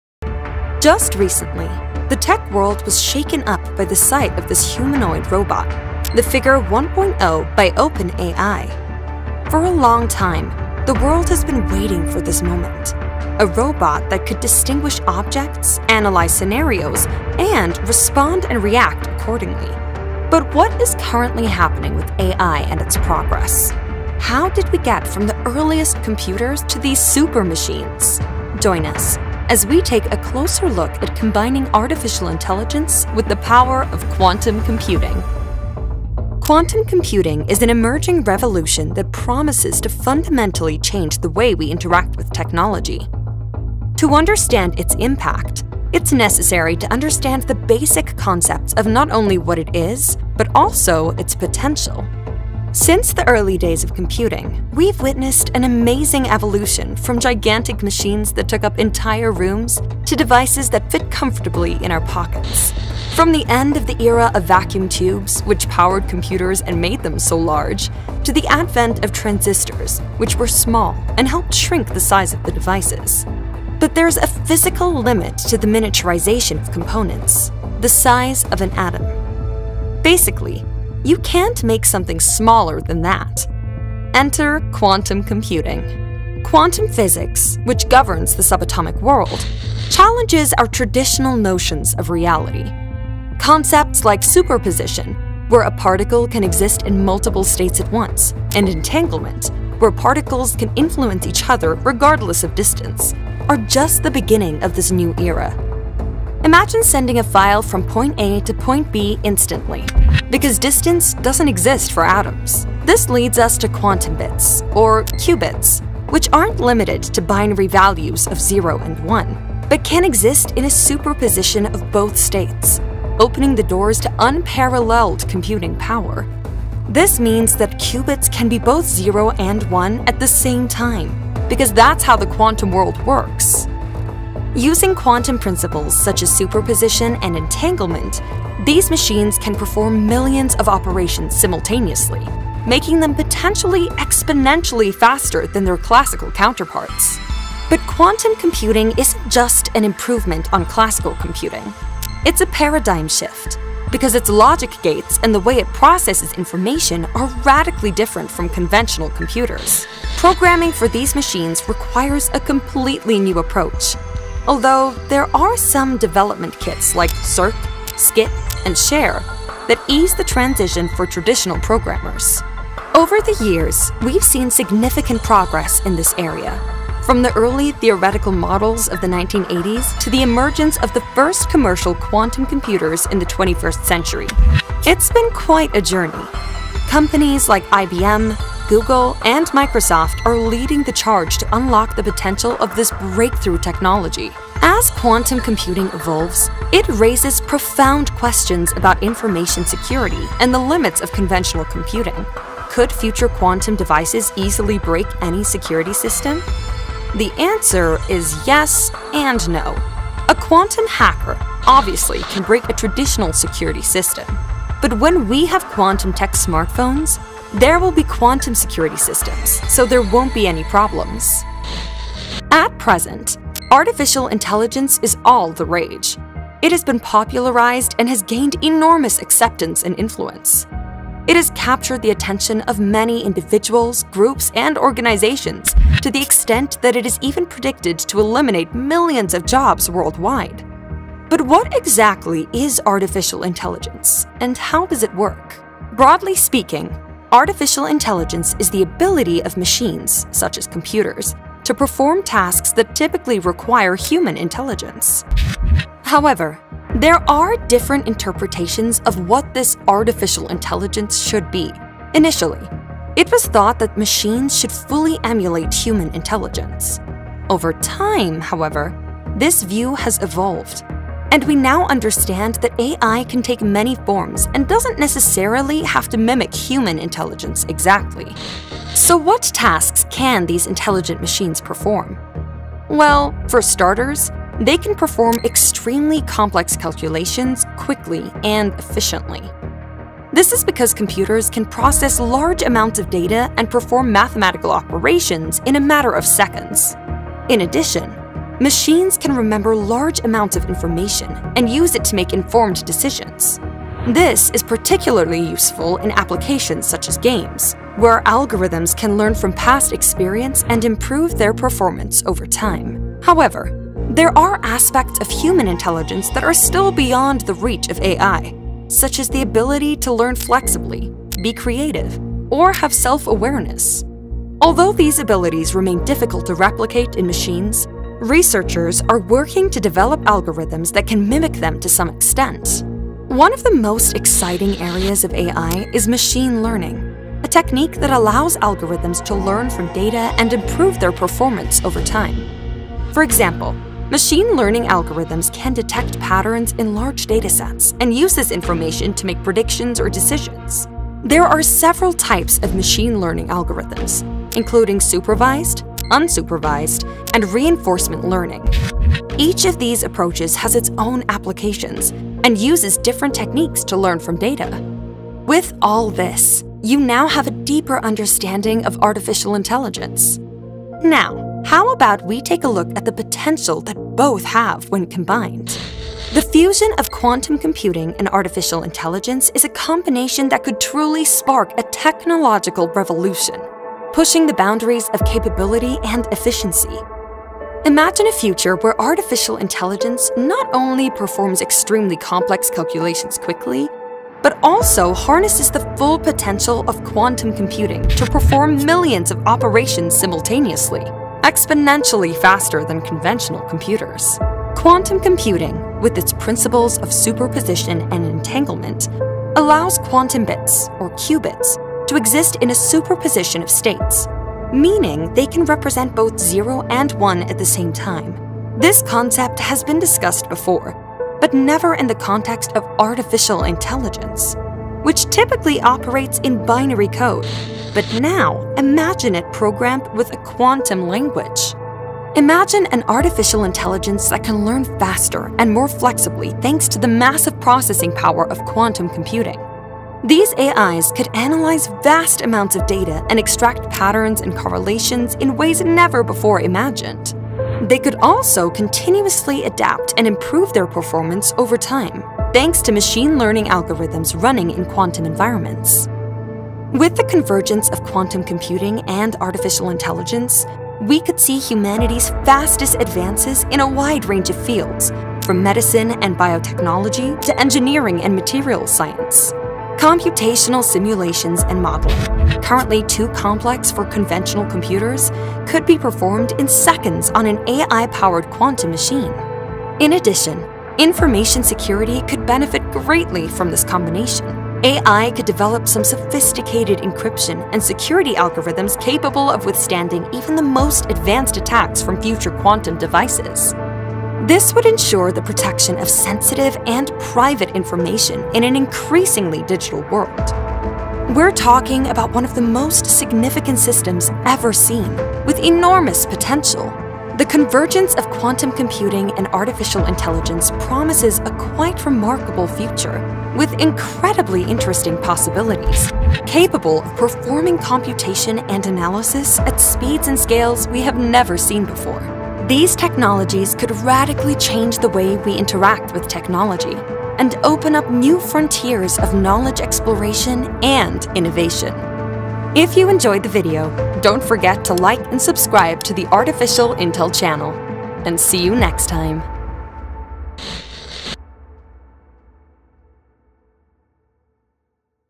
The INSANE Power of Quantum Artificial Intelligence
What is currently happening with AI and its progress? How did we get from the earliest computers to these super machines? Join us as we take a closer look at combining artificial intelligence with the power of quantum computing. A collision of two technologies that could become the next big tech revolution. A quantum computer could potentially be a million times faster than currently existing supercomputers. When combined with AI, the possibilities are endless...